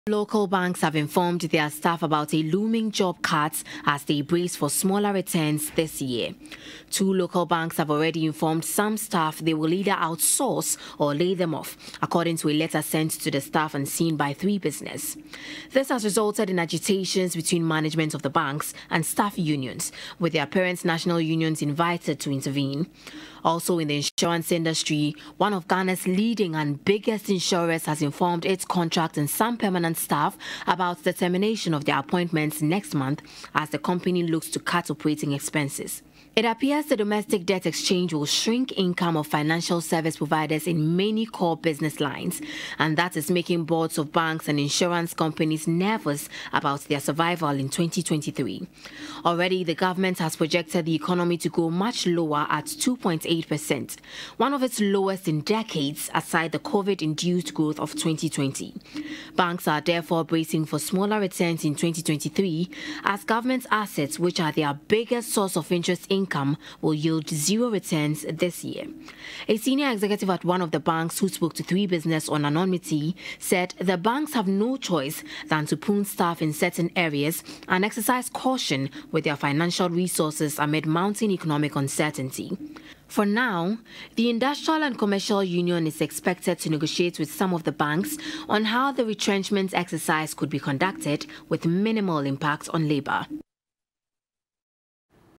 Financial institutions layoff: Some banks, insurance companies begin to downsize.
Some banks and insurance companies have begun laying off workers in anticipation of a difficult year for the financial sector.